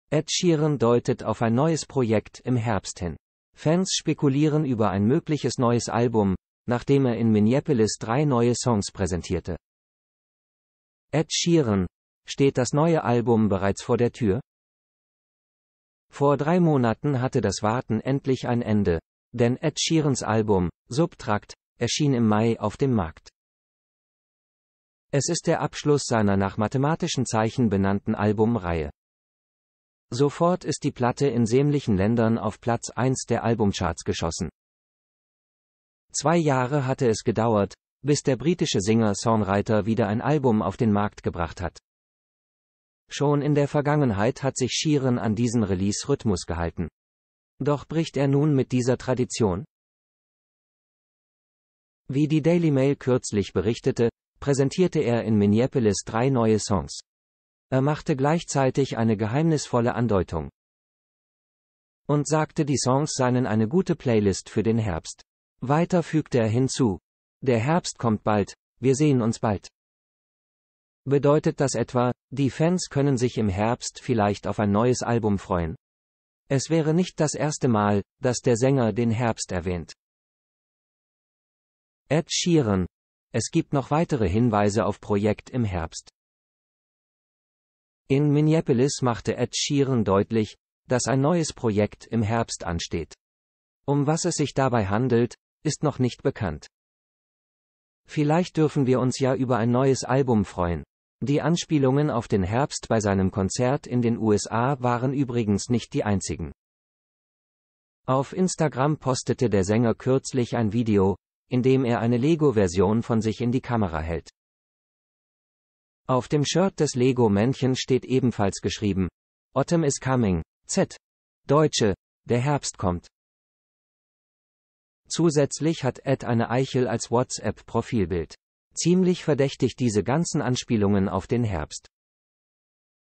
Ed Sheeran Geheimnisvolle Andeutung Steckt ein neues Album dahinter
Ed Sheeran deutet auf ein neues Projekt im Herbst hin. Fans spekulieren über ein mögliches neues Album, nachdem er in Minneapolis drei neue Songs präsentierte.